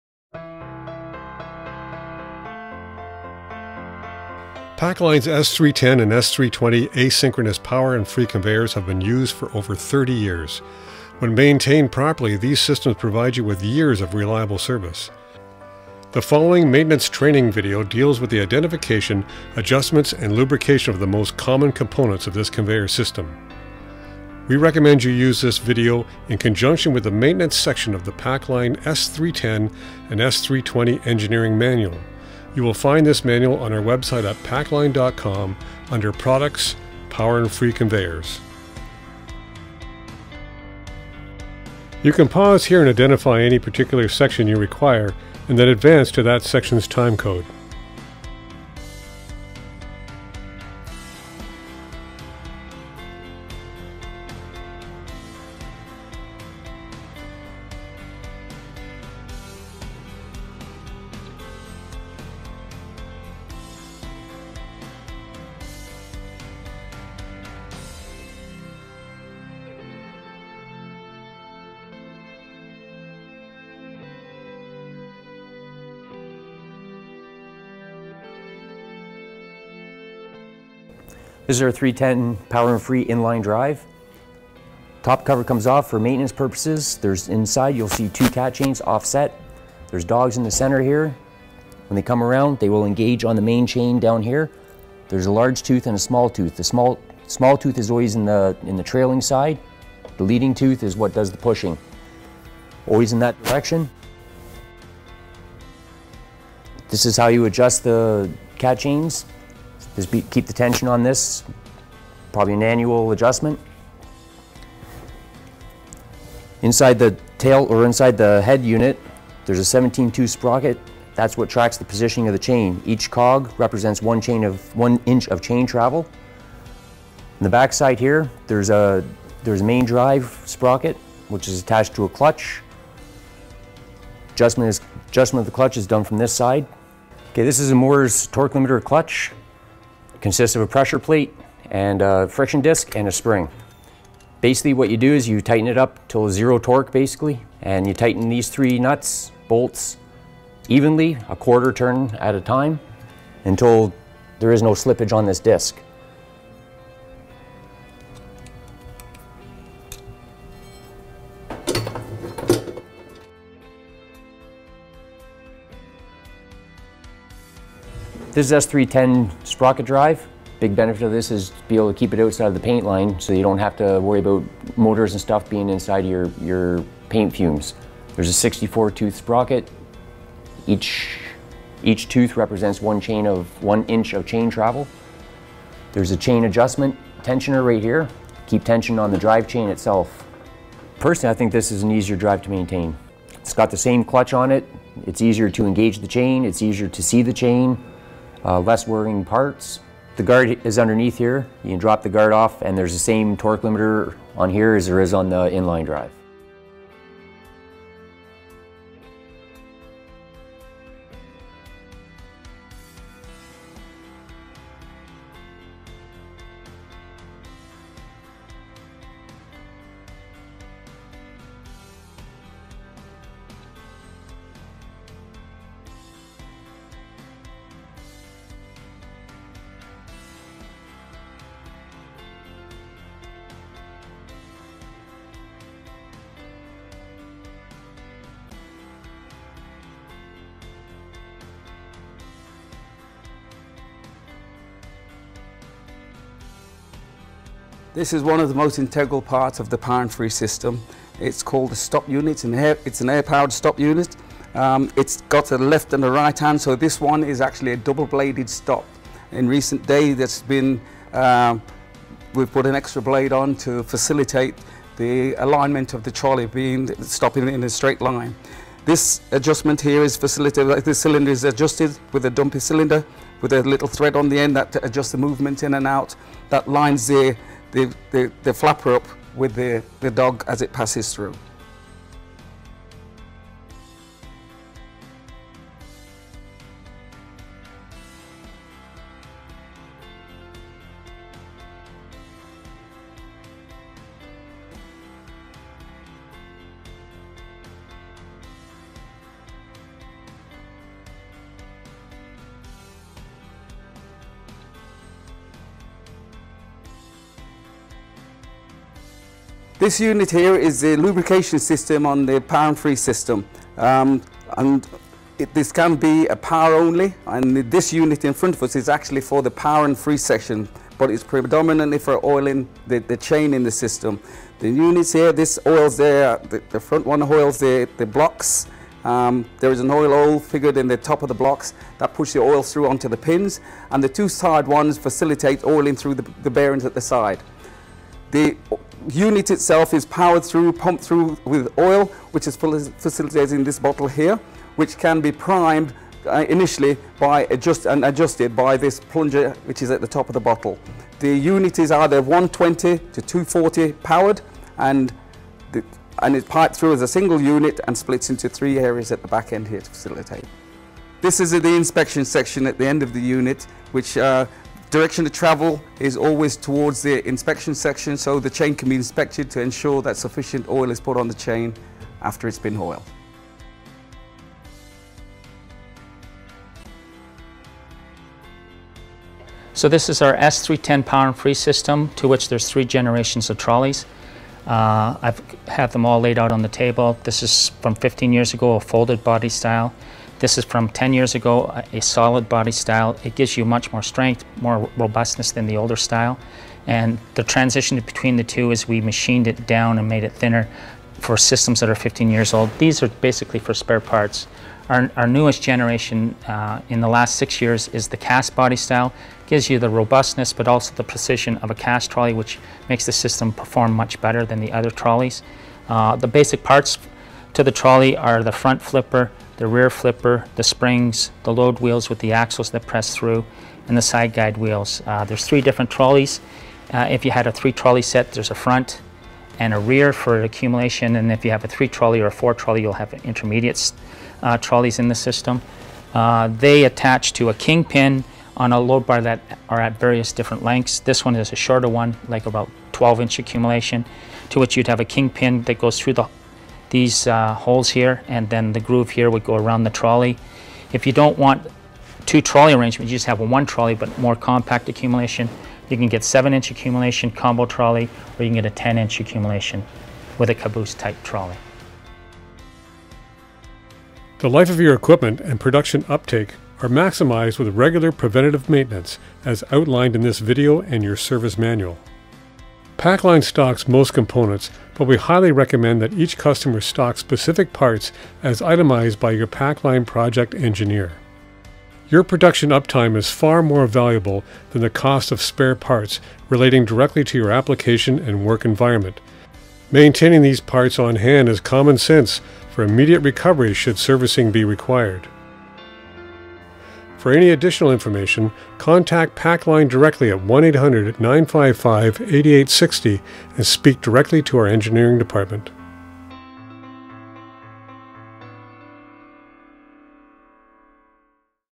Power and Free Conveyor Maintenance Video
This video was prepared to work in conjunction with our manual. The combination of the video with the manual will enhance your instructional training for installation and maintenance of your Power and Free Pacline conveyor.

Please download your copy of our PACLINE Power and Free Conveyor Installation and Maintenance Manual